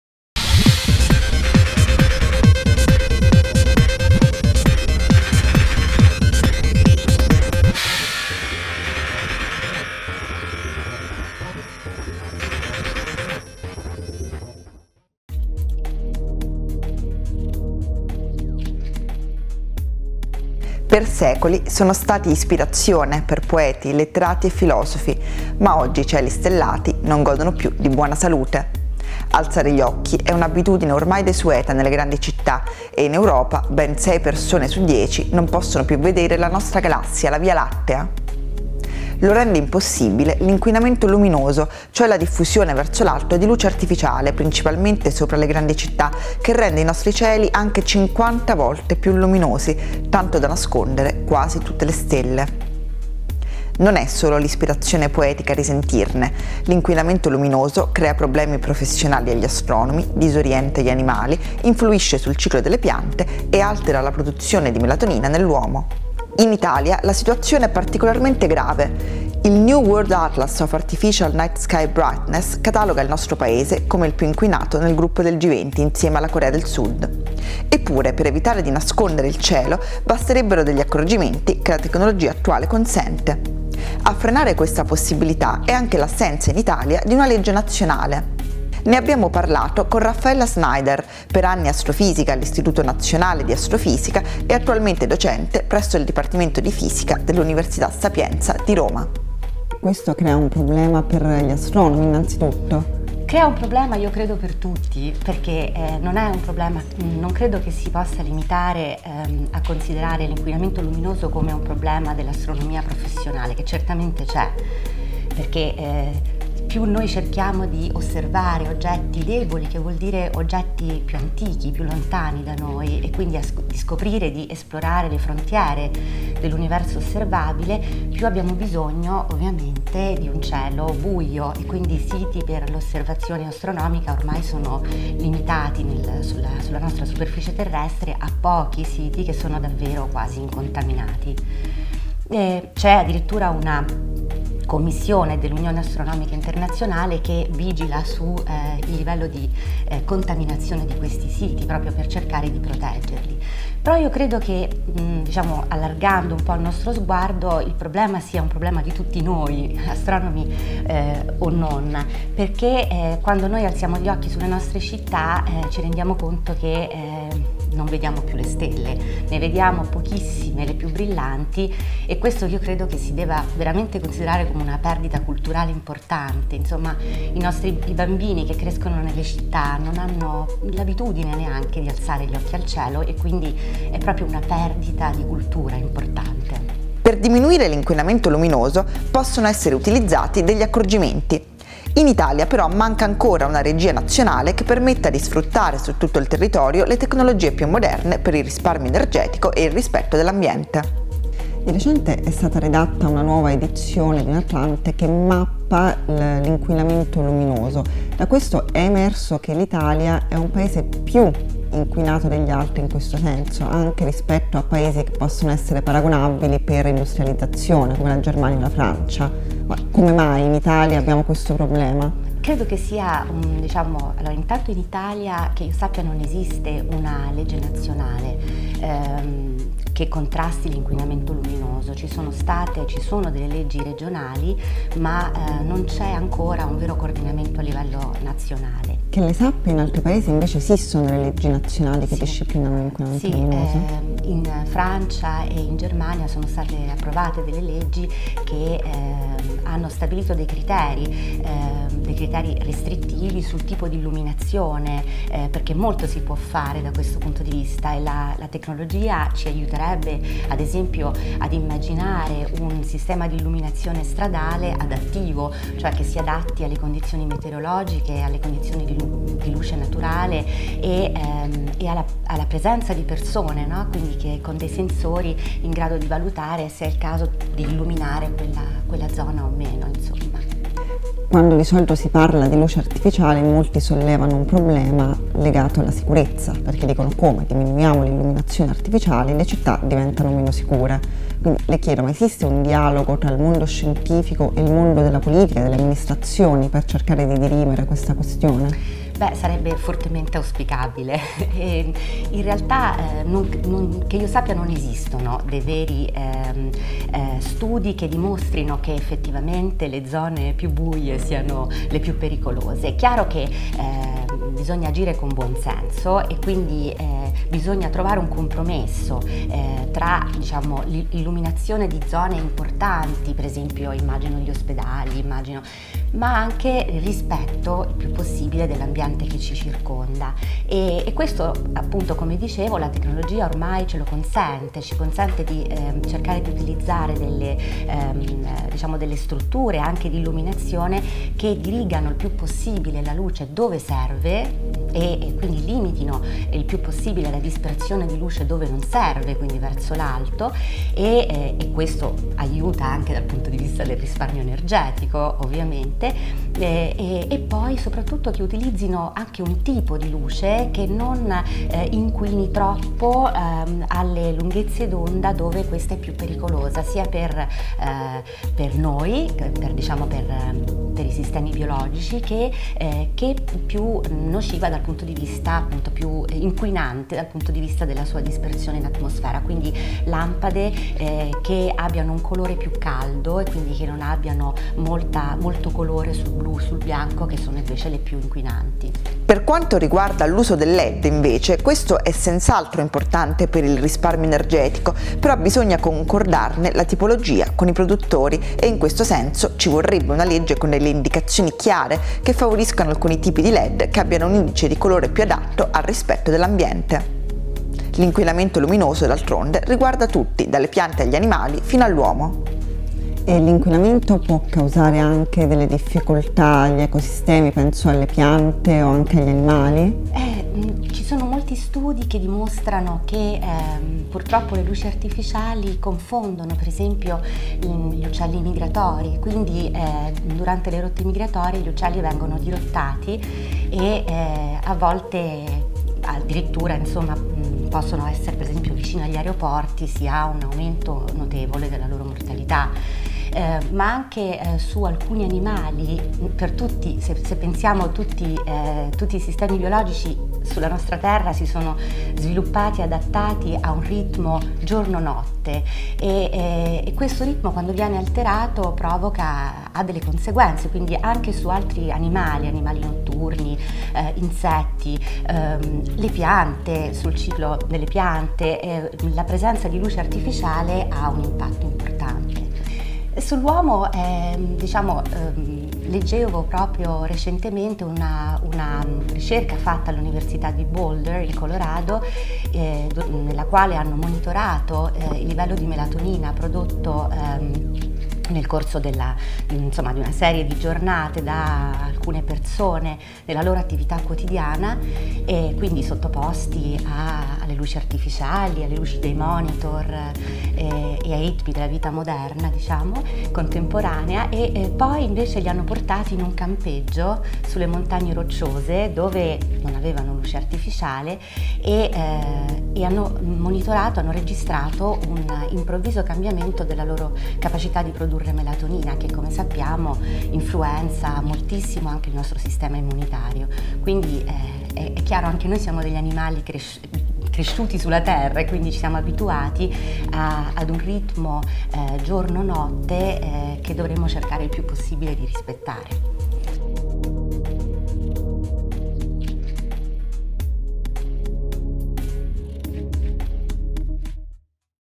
SCIENTIFICAMENTE: Troppa luce fa male, ecco i danni dell'inquinamento luminoso
Per secoli sono stati ispirazione per poeti, letterati e filosofi, ma oggi i cieli stellati non godono più di buona salute. Alzare gli occhi è un'abitudine ormai desueta nelle grandi città e in Europa ben sei persone su dieci non possono più vedere la nostra Galassia, la Via Lattea.

Ce ne parla Raffaella Schneider, astrofisica - Università 'Sapienza' di Roma